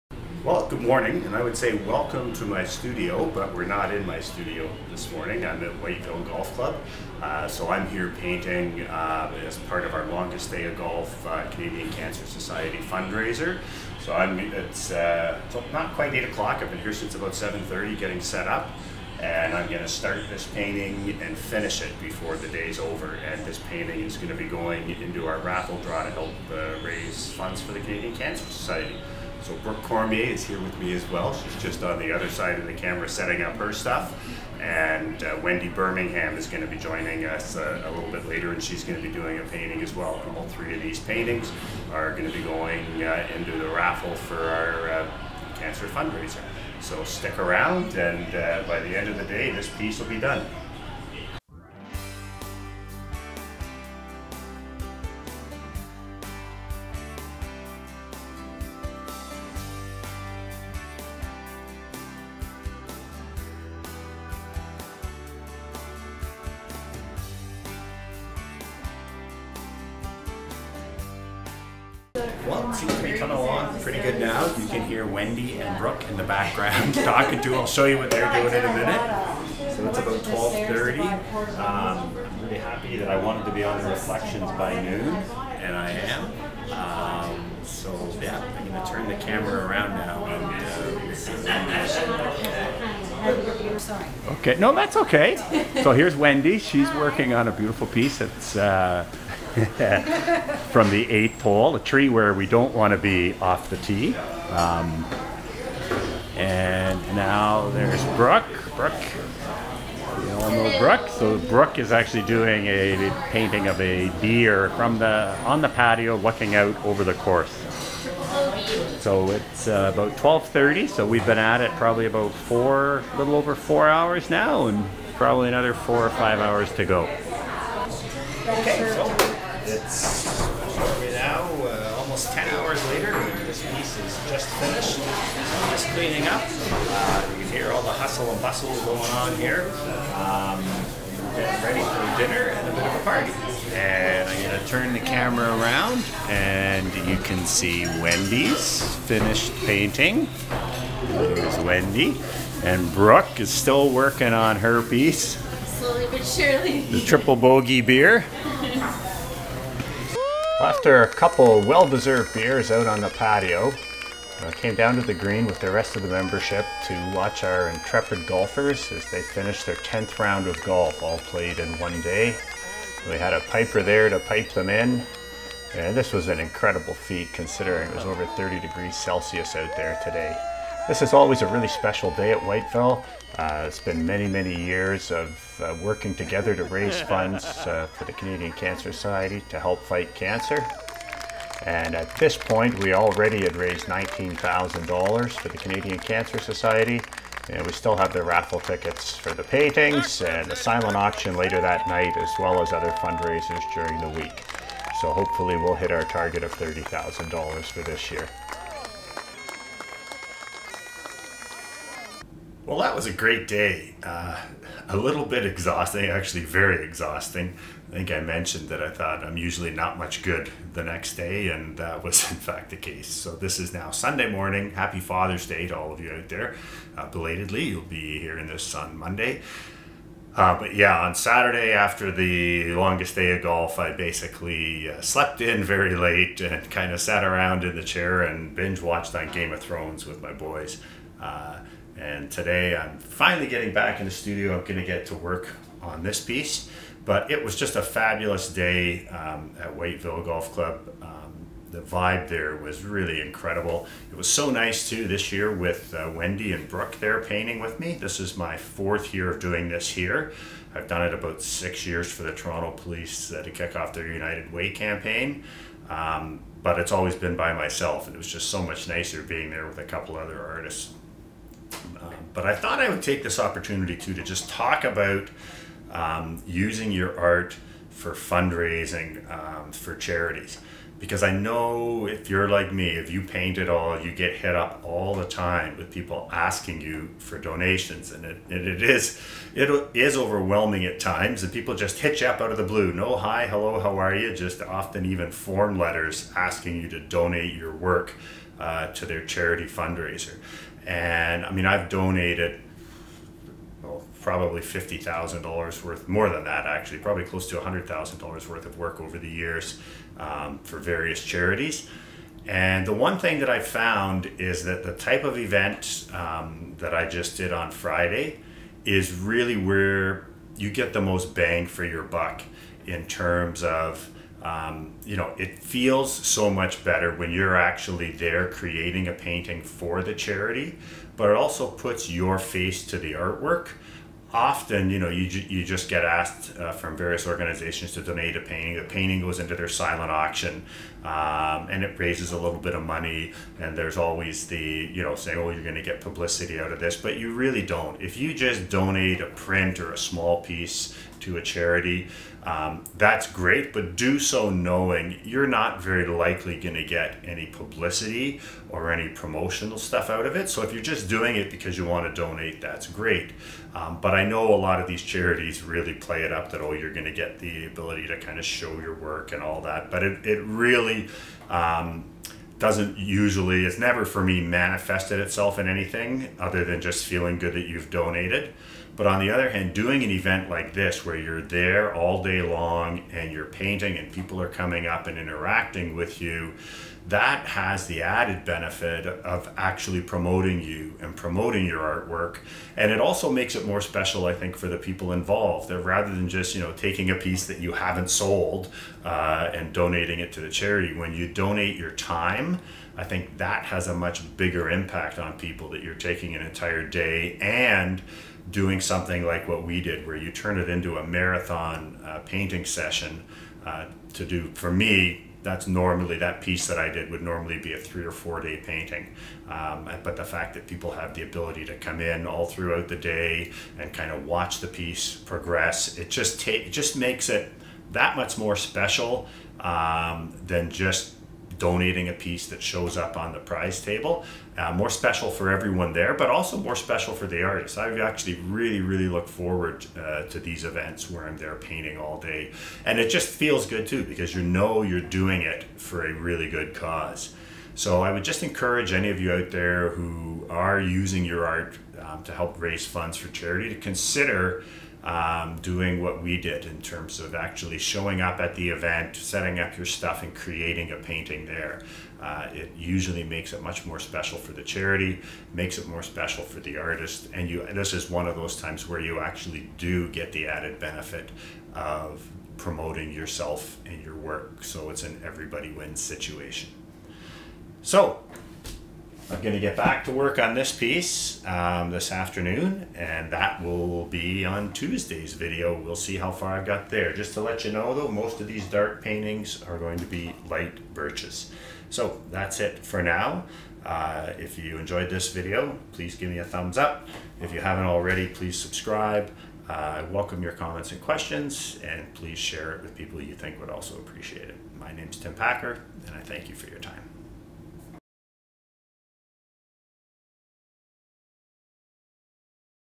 A Marathon Painting Day
I complete a 30" by 40" painting in one day to help raise funds for Cancer Research.